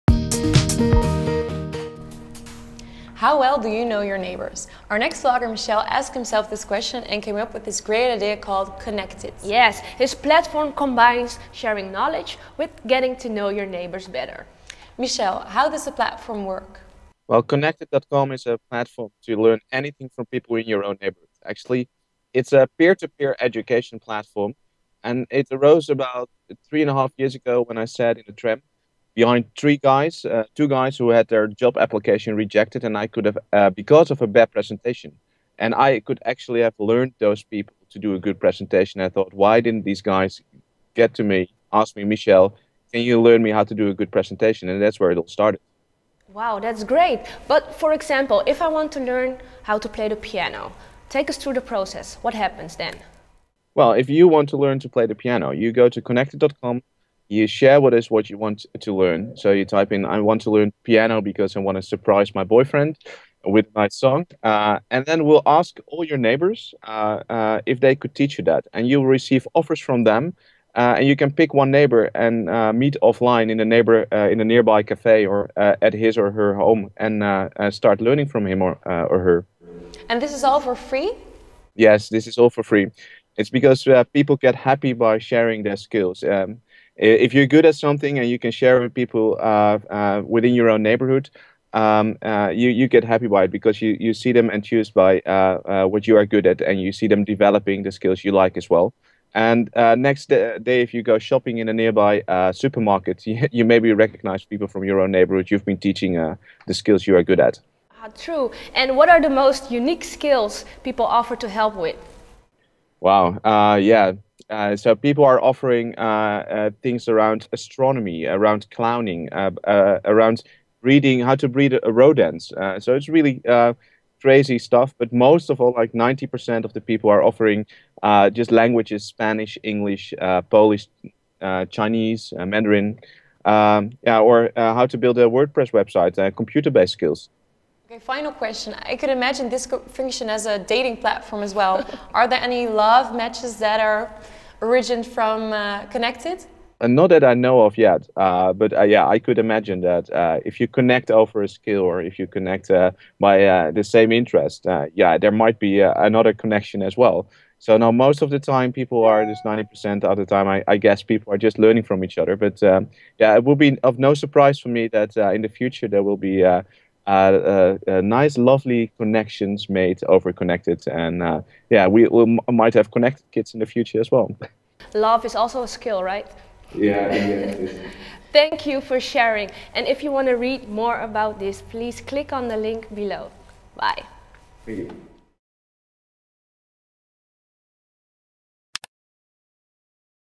How Neighbours Can Be The Solution To Your Problems
Almost 70% of the people lives in cities and that makes us surrounded by skills and knowledge. Konnektid is the first demand-based platform of the Netherlands where people can share skills and knowledge with each other. The platform connects people that are nearby each other that offer or search for a certain skill. Michel, the founding father of this simple but genius concept, joins IntoConnection in this two minute video to talk about what they exactly do.

Private users
Konnektid is mainly focused on individuals using the platform for several ends. Whether you want to learn to play an instrument or if you are looking for someone to teach you how to cook a lovely dinner, think of a question and you can ask it on the platform! Thereby, Konnektid makes many seemingly difficult issues solvable with the knowledge available within your own neighbourhood.

How they earn their money
The service they offer their users is for free and the startup is able to do so as the money they earn comes from the business side. Companies that organise events and conventions can make Konnektid-groups for their visitors but they have to pay for that. Within the group the visitors can ask questions, deliberate and exchange knowledge.

Interested in this platform? Make sure to connect with Michel!